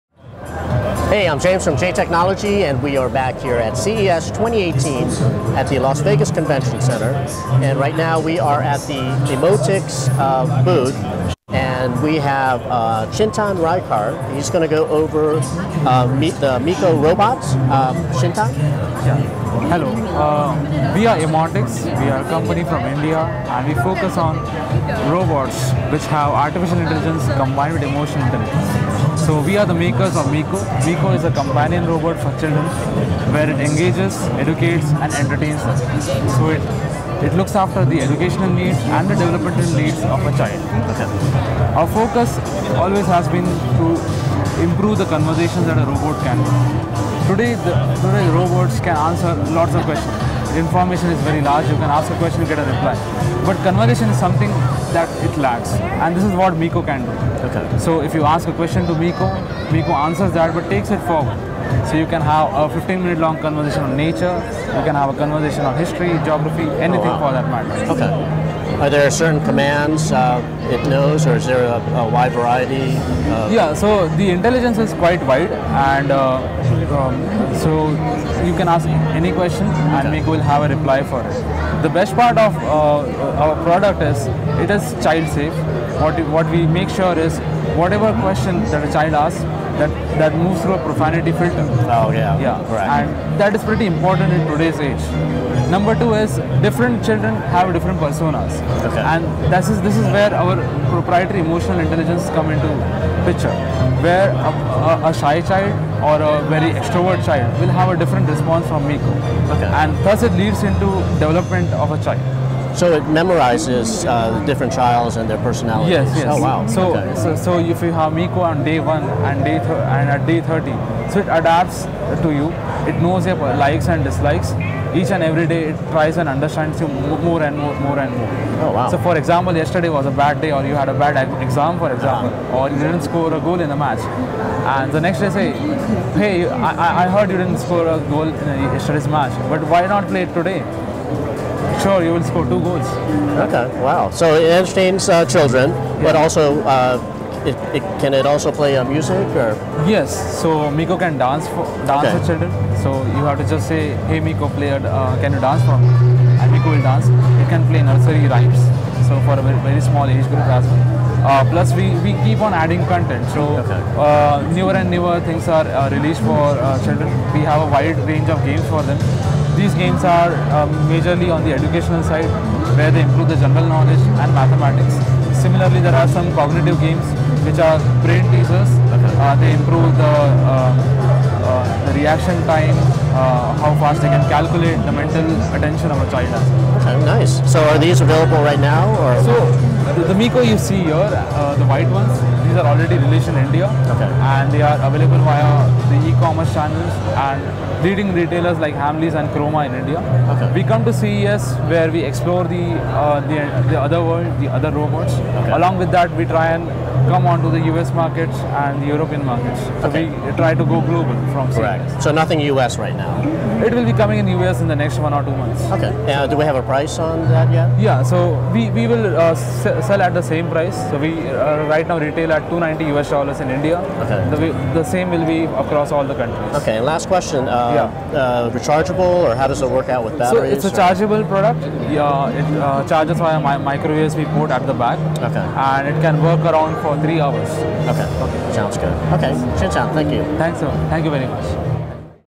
Emotix Miko: My Companion Robot at CES 2018
Interview with Chintan Raikar on the Emotix Miko: My Companion Robot at CES 2018.

Host: James Valle
Director & Producer: Mark L. Valle
Cameraman: Mark L. Valle
Edited by: Mark L. Valle